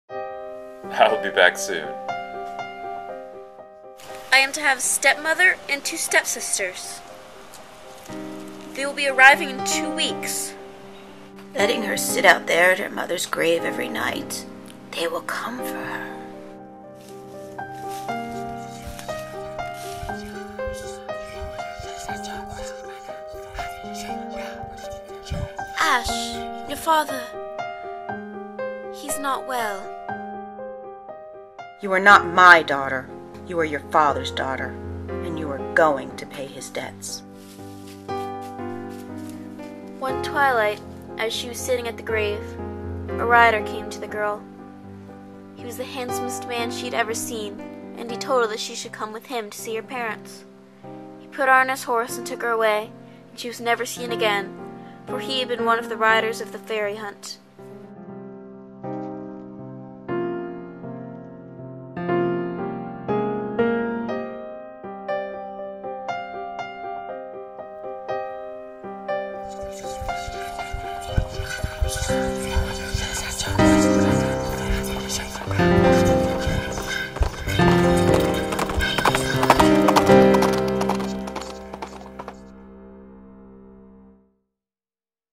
"Ash" Book trailer
Trailer for the book "Ash," by Malinda Lo. Entry for YALSA book contest as Morris finalist.